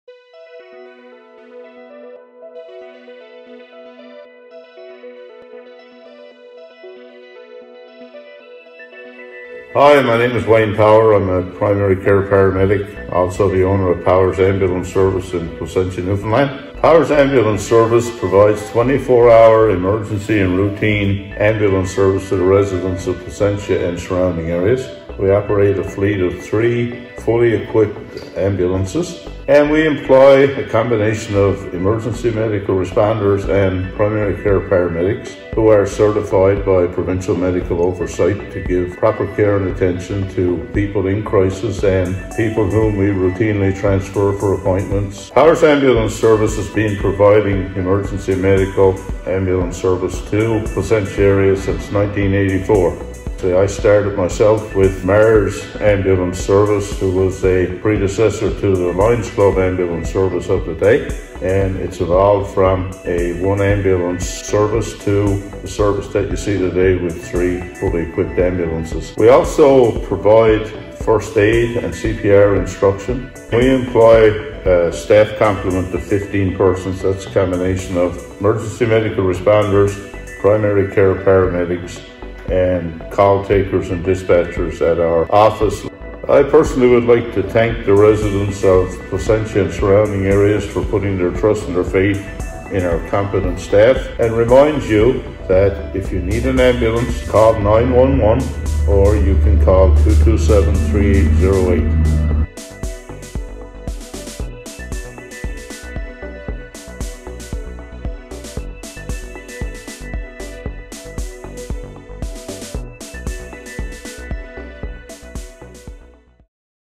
Spotlight Series: Power's Ambulance Service Ltd.
The Placentia Chamber of Commerce is proud to present our "Bridging Community and Business" Spotlight Series, as we highlight businesses who are members of the Chamber.

Placentia Chamber of Commerce:

9:00am-4:30pm
Monday to Friday
Located in the Placentia Mall

Shot and Edited by Maggie Follett
Additional stock footage from Pexels.